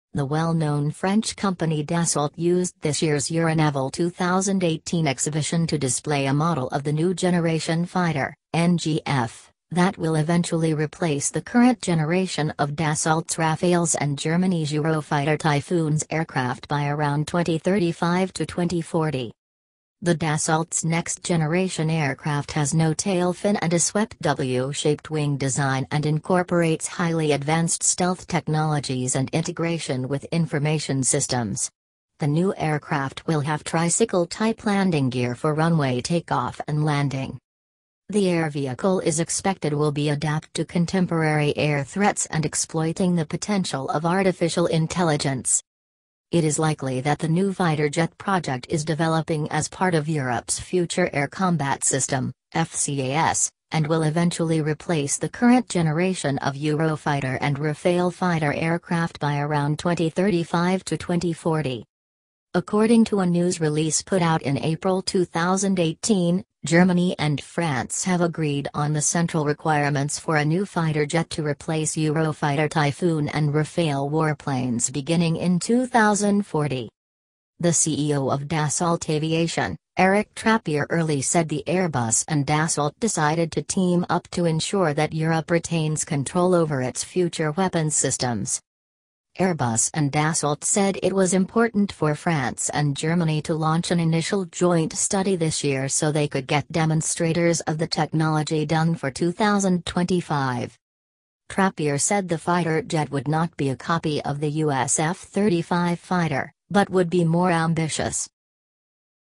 France Unveils Model of the New Generation Fighter
French company Dassault displays a model of the New Generation Fighter (NGF) at Euronaval 2018 exhibition, that will eventually replace the current generation of Dassault’s Rafales and Germany’s Eurofighter Typhoons aircraft by around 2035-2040.